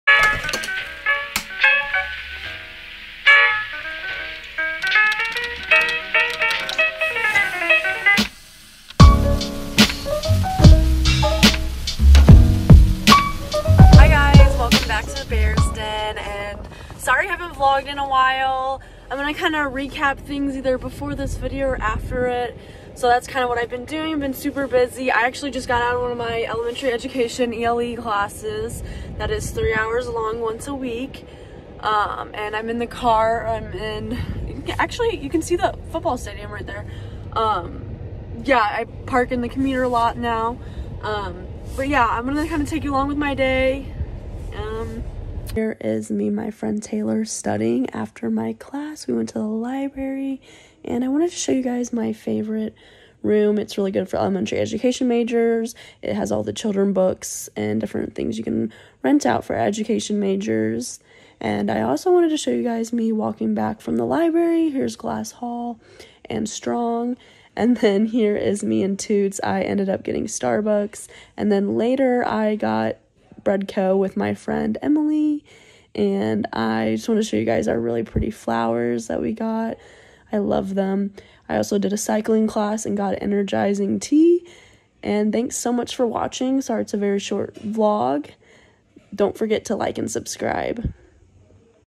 Day in the Life of a College Student at Missouri State University
Hey Friends!! In this video, I film what a day in my life as a college student at Missouri State University looks like. I am currently a Junior at Missouri State University and I am majoring in Elementary Education. I hope you enjoy!

Learn more about Missouri State University at

The Bears Den is a vlog series featuring student life at Missouri State University.

xoxo,
Madi